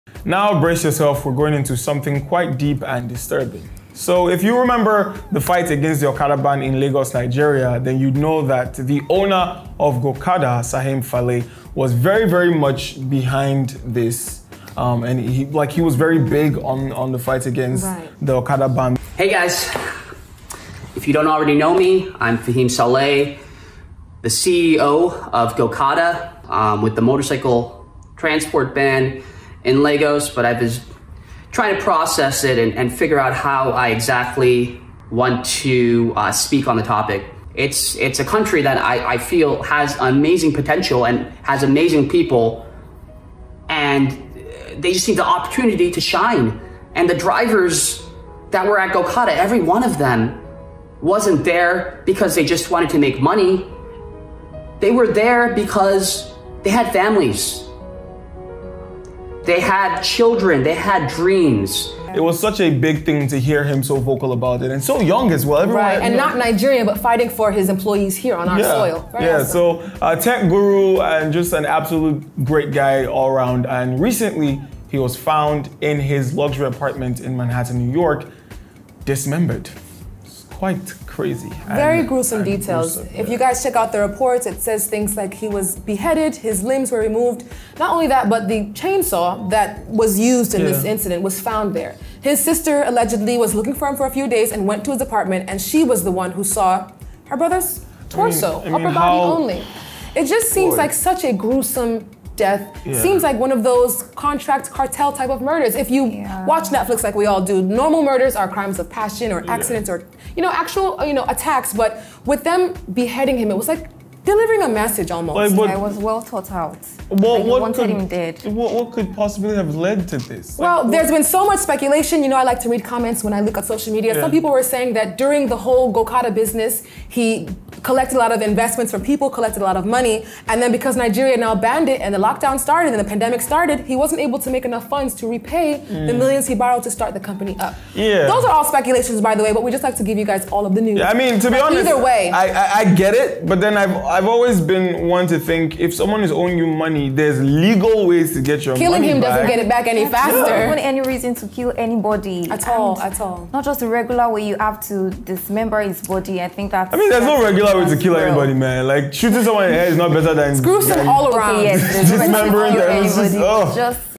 Gokada's CEO, 33Yr Old, Fahim Saleh Murdered and Butchered In New York
This news of the inhumane murder of Gokada CEO left many of us devastated!